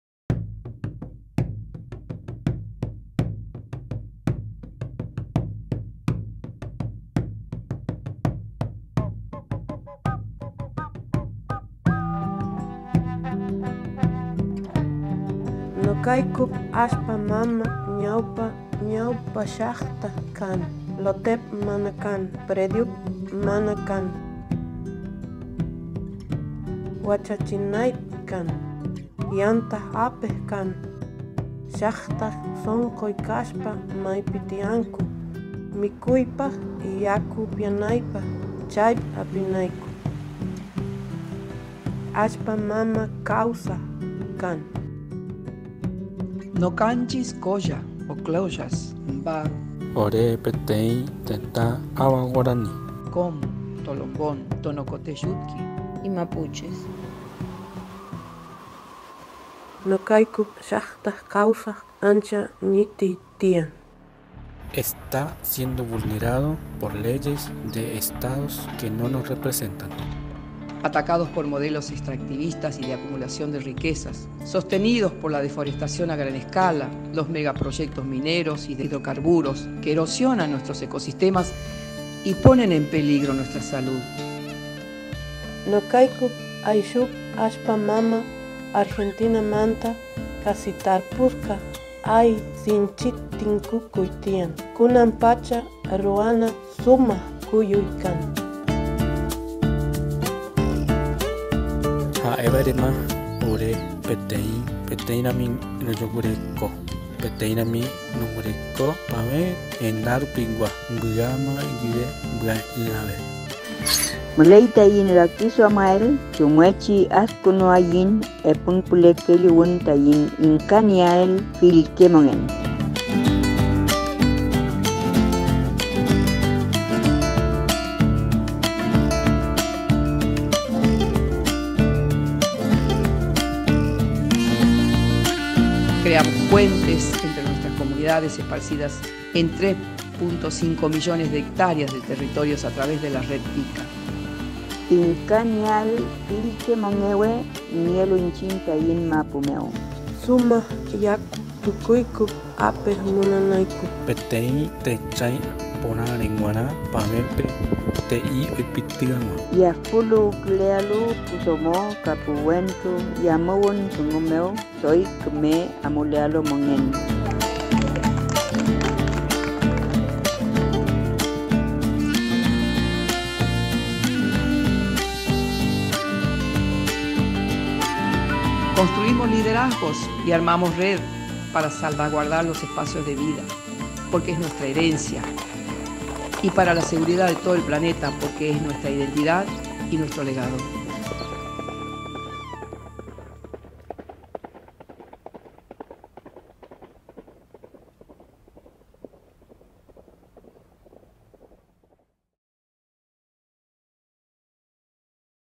RED TICCA "Territorios de Vida en Argentina" - Winner of the Equator Prize 2022 from Argentina
The ICCA Network 'Territories of Life in Argentina' (RED TICCA) is an autonomous political organization of Indigenous peoples for the recognition and application of Indigenous rights and the defense of their territories at the national level. RED TICCA’s work promotes political recognition of Indigenous peoples as bearers of a millenary wisdom regarding the protection of nature and ancestral guardians of the territory.

The network is groundbreaking in its ability to bridge communities throughout the country to achieve food sovereignty, access to clean water, improved public health, intergenerational equity and gender equality. The communities have also protected their ancestral lands by joining the global ICCA Consortium, a global movement to protect the territories of Indigenous Peoples and local communities (a.k.a “Territories of Life”). RED TICCA has connected and empowered the Indigenous peoples of Argentina and has reaffirmed their ownership over their territories, culture, and natural resources - a historic step for Indigenous rights and recognition in Argentina.